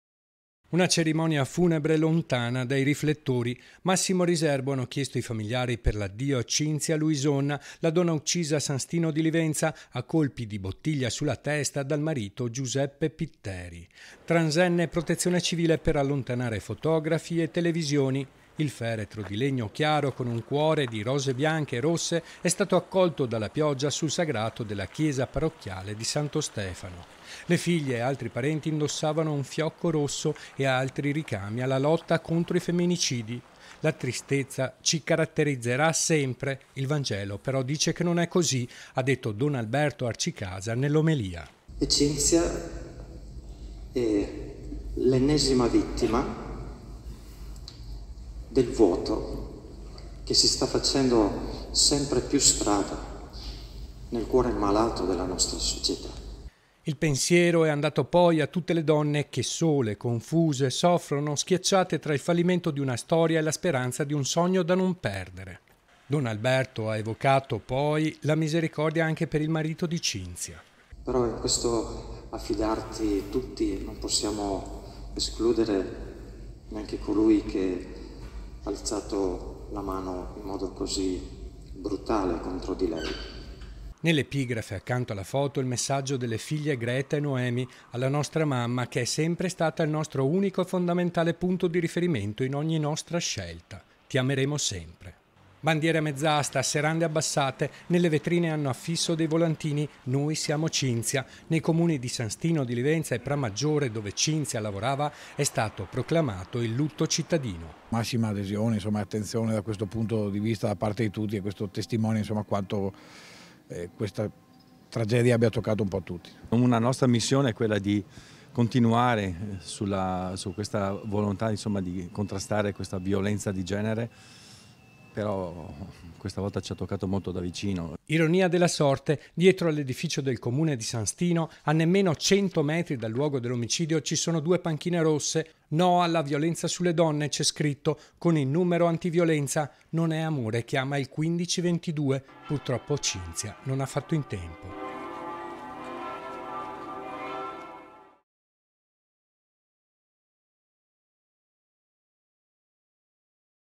L'ULTIMO SALUTO A CINZIA: «ENNESIMA VITTIMA DI UNA PIAGA DILAGANTE» | 16/12/2022
A3 NEWS Veneto 16/12/2022 - SANTO STINO DI LIVENZA - Si è celabrato questa mattina a San Stino di Livenza il funerale di Cinzia Luison, ennesima vittima di femminicidio. «Ennesima vittima di una piaga dilagante» ha detto don Alberto Arcicasa nell'omelia.  || Una cerimonia funebre lontana dai riflettori. Massimo riserbo hanno chiesto i famigliari per l'addio a Cinzia Luison, la donna uccisa a san Stino di Livenza a colpi di bottiglia sulla testa dal marito Giuseppe Pitteri. Transenne e Protezione Civile per allontanare fotografi e televisioni. Il feretro di legno chiaro con un cuore di rose bianche e rosse è stato accolto dalla pioggia sul sagrato della chiesa parrocchiale di Santo Stefano. Le figlie e altri parenti indossavano un fiocco rosso e altri richiami alla lotta contro i femminicidi. «La tristezza ci caratterizzerà sempre. Il Vangelo però dice che non è così», ha detto don Alberto Arcicasa nell’omelia. Il pensiero è andato poi, a tutte le donne, che sole, confuse, soffrono, schiacciate tra il fallimento di una storia e la speranza di un sogno da non perdere.Don Alberto ha evocato poi la misericordia anche per il marito di CinziaNell'epigrafe accanto alla foto il messaggio delle figlie Greta e Noemi: Alla nostra mamma che è sempre stata il nostro unico e fondamentale punto di riferimento in ogni nostra scelta: ti ameremo sempre. Bandiere a mezz'asta, serrande abbassate. Nelle vetrine hanno affisso dei volantini, noi siamo Cinzia. Nei comuni di San Stino di Livenza e Pramaggiore dove Cinzia lavorava, è stato proclamato il lutto cittadino.Ironia della sorte, dietro all'edificio del Comune di San Stino, a nemmeno cento metri dal luogo dell'omicidio, ci sono due panchine rosse, No alla violenza sulle donne c'è scritto, con il numero anti violenza, non è amore chiama il 15 22. Purtroppo Cinzia non ha fatto in tempo. - Intervistati MATTEO CAPPELLETTO (Sindaco di San Stino di Livenza), FAUSTO PIVETTA (Sindaco di Pramaggiore) (Servizio di Nicola Marcato) - Segui Antenna Tre anche sul digitale terrestre! Visita il sito